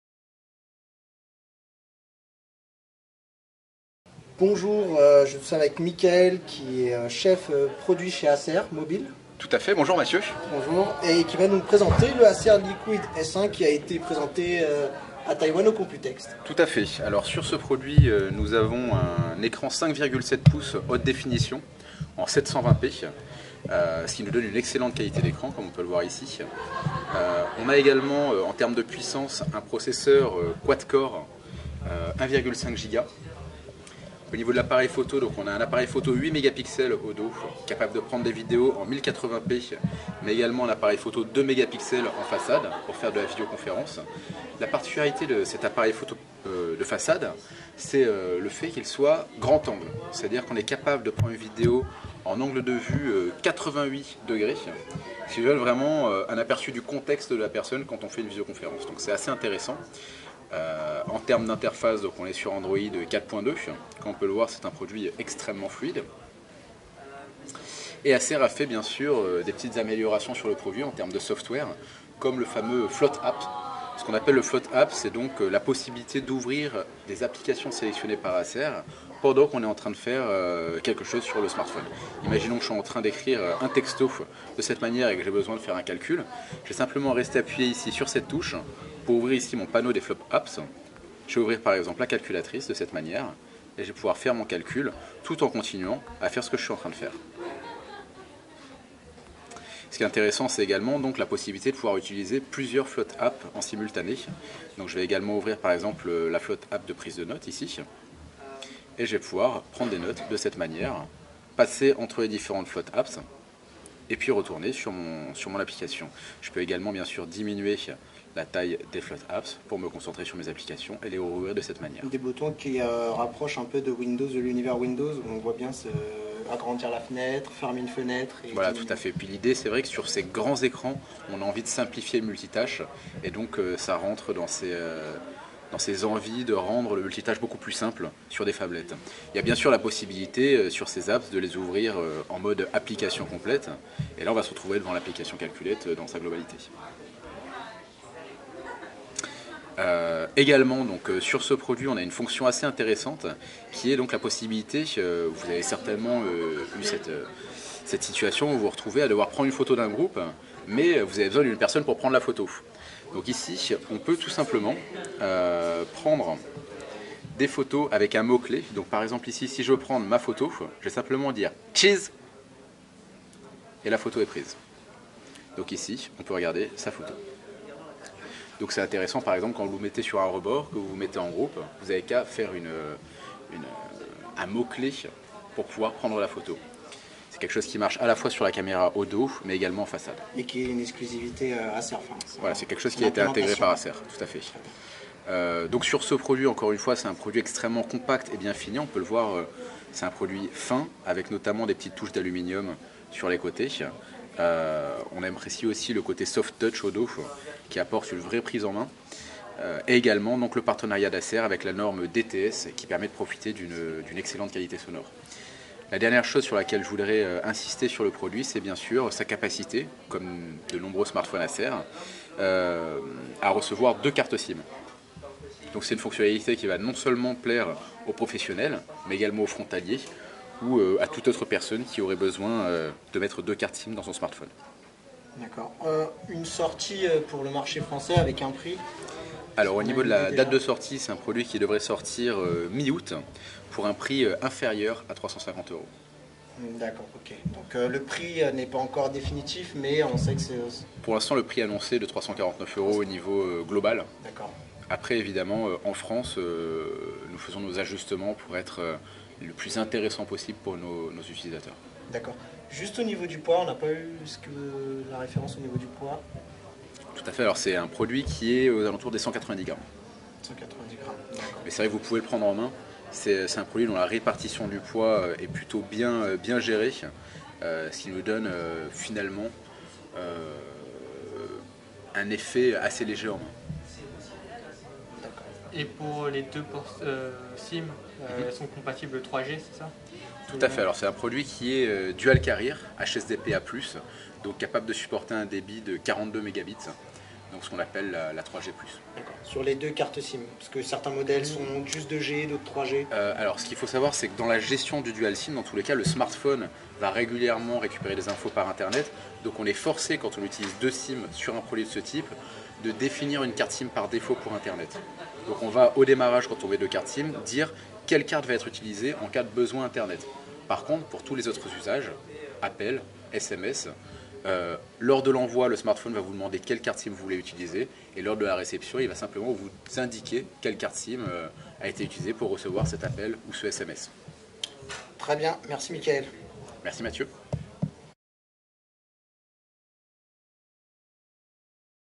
Acer Liquid S1 Présentation vidéo Communaute-Android
A découvrir notre prise en main du Acer Liquid S1, annoncé lors du Computex de Taïwan. Acer France a présenté la nouvelle phablet lors d'un showroom à Paris